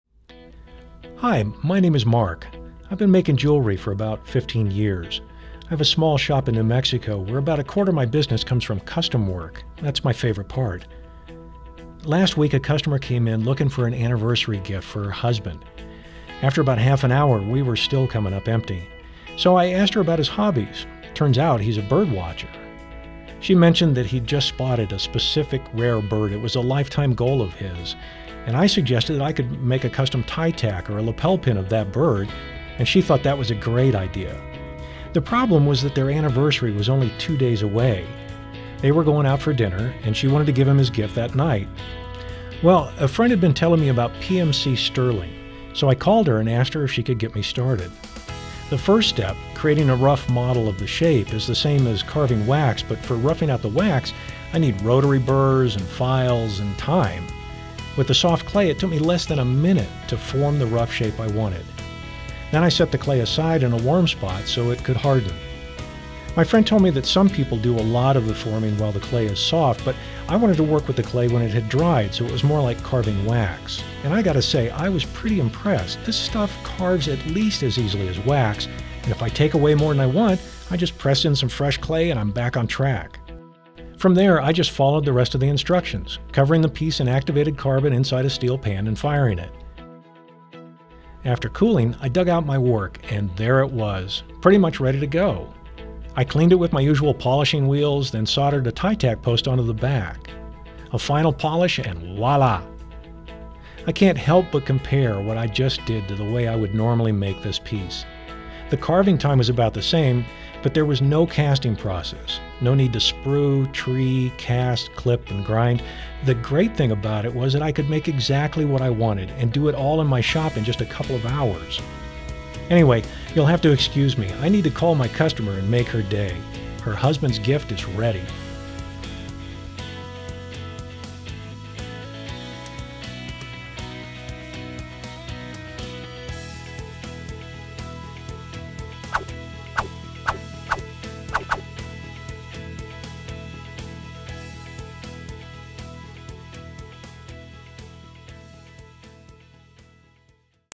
Using PMC Sterling for Lapel Pins
Learn what makes PMC sterling truly special and how it can expand your project capabilities with less time and tools. PMC sterling is ideal for custom works, pleasing jewelers, and their clients for its ability to create stunningly unique pieces - even on short notice. Unlike working with wax, your need to cast, mold, and grind is eliminated from the process. Only a few hours is all it takes to create unique stunning sterling silver pieces with PMC sterling.

Foredom® SR Motor with H.30 Handpiece Flex Shaft System with Motor Hanger